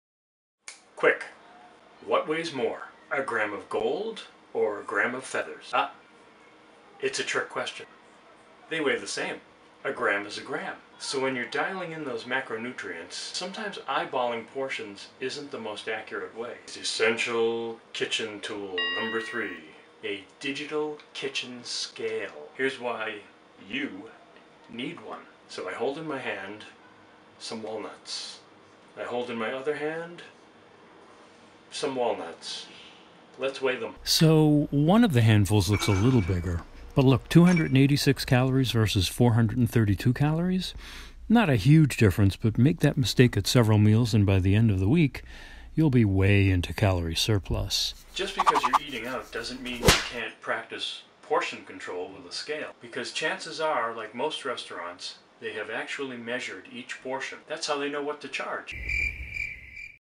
NO WEIGH. YES, WEIGH
There are a lot of people who cannot abide the thought of weighing their portions. Here is my 59.2 second video argument in favor, especially if you’re chasing a six pack.
Episode 3 of a 7 part series highlighting 7 Essential Kitchen Tools illustrates how easy it is innocently overestimate your calories if you only “eyeball” portions..
Need help setting up your kitchen and yourself for six pack success?